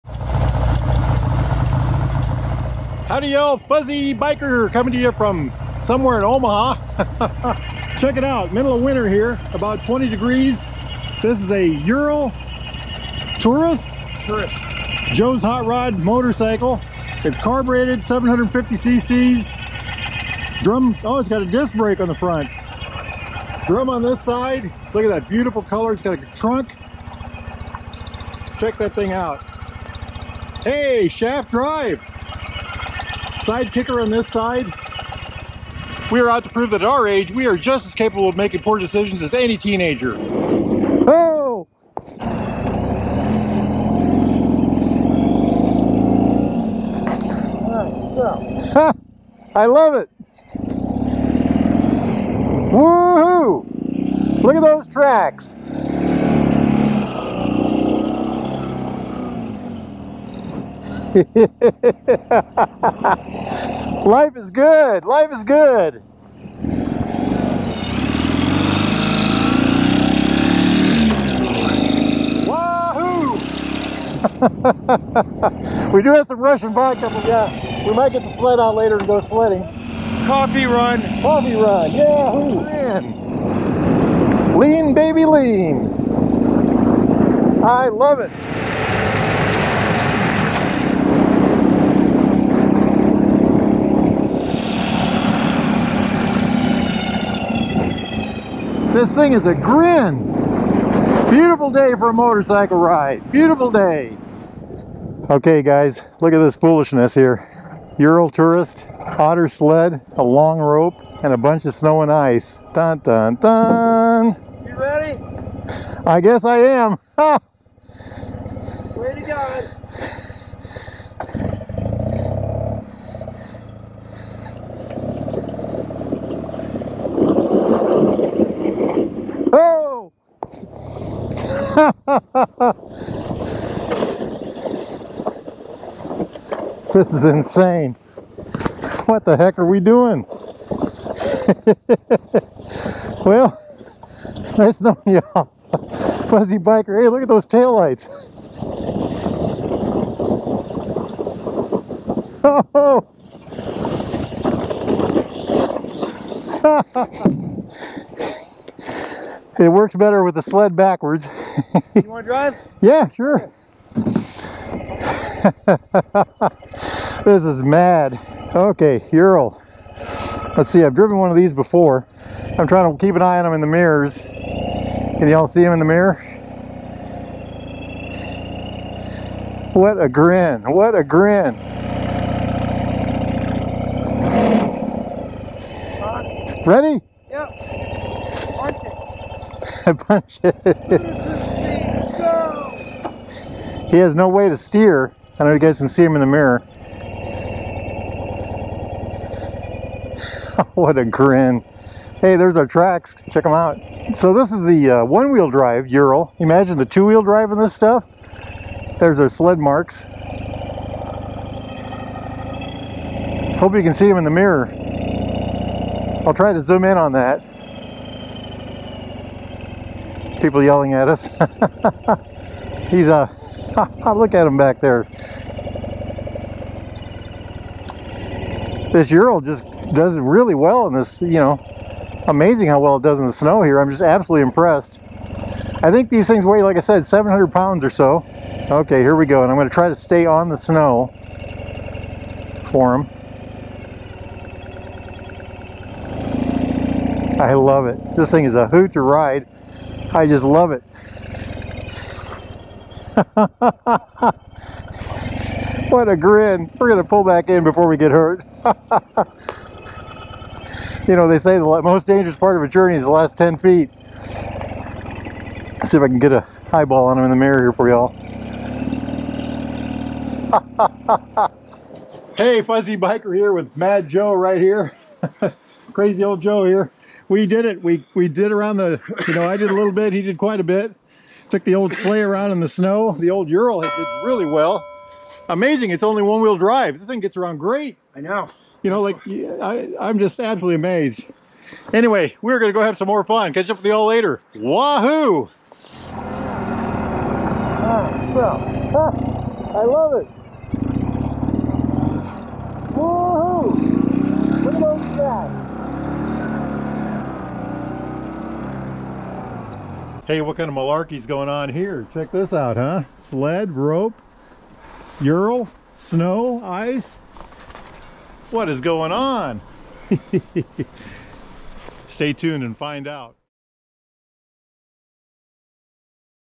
Crazy Joe's Bright Red Ural Tourist - True Winter Fun Machine - Wahoo!
Our good friend Joe knows how to do it. Watch for more motorcycle madness to come. Did I mention he pulls his sailboat with a Tenere? Wahoo!